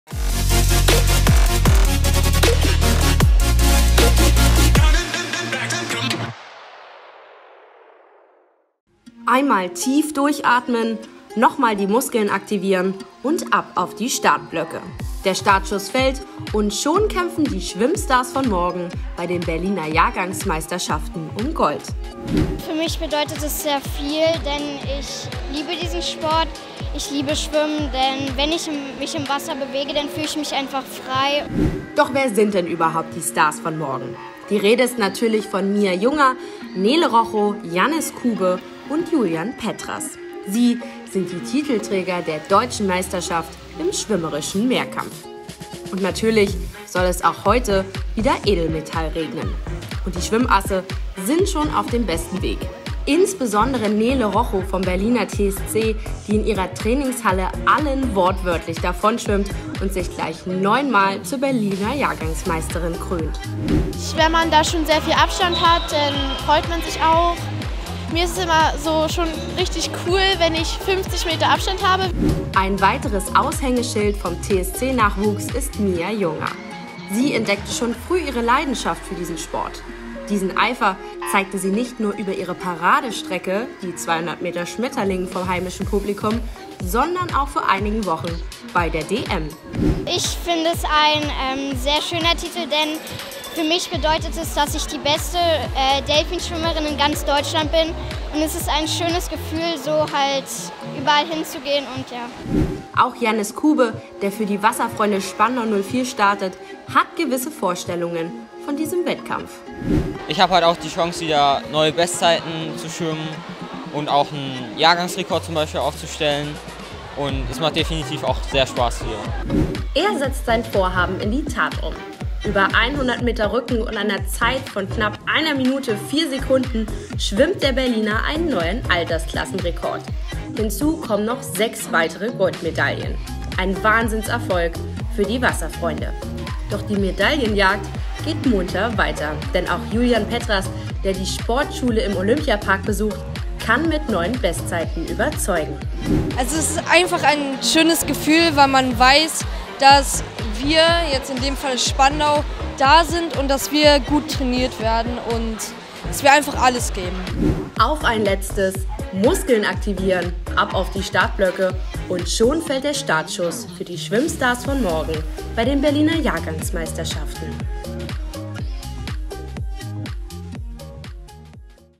DIE STARS VON MORGEN | Berliner Jahrgangsmeisterschaften und Meisterschaften
Einmal tief durchatmen, noch mal die Muskeln aktivieren und ab auf die Startblöcke. Der Startschuss fällt und schon kämpfen die Schwimmstars von morgen bei den Berliner Jahrgangsmeisterschaften um Gold.